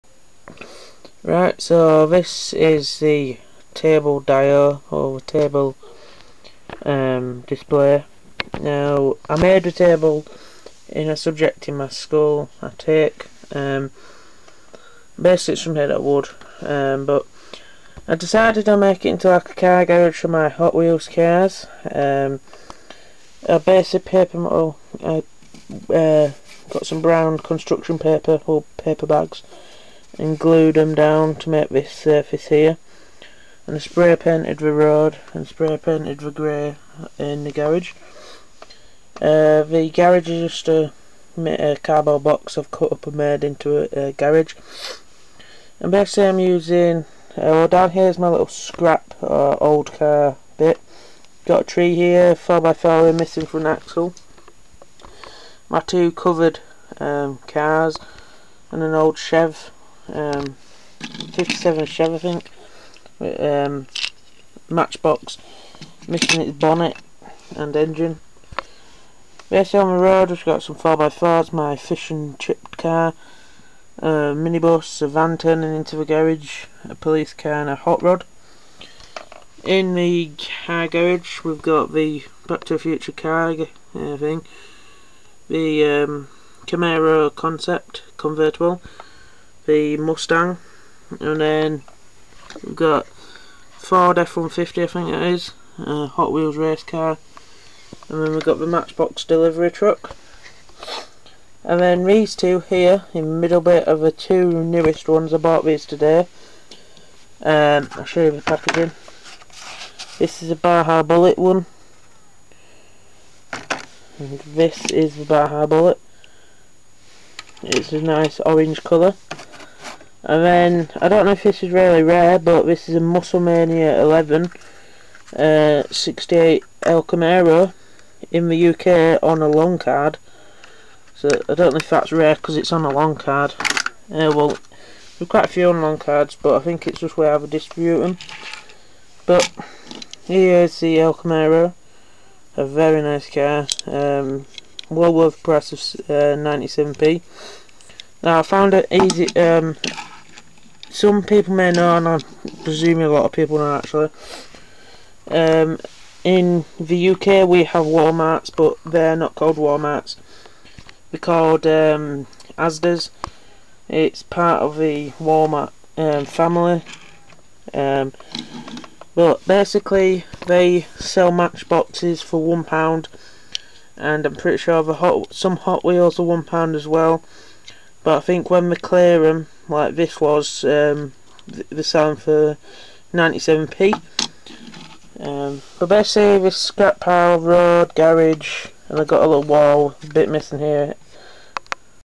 Car Garage Dio
Well i Made the Table at School and Made a 1/64th Car Garage on top of it.
so hope you enjoy.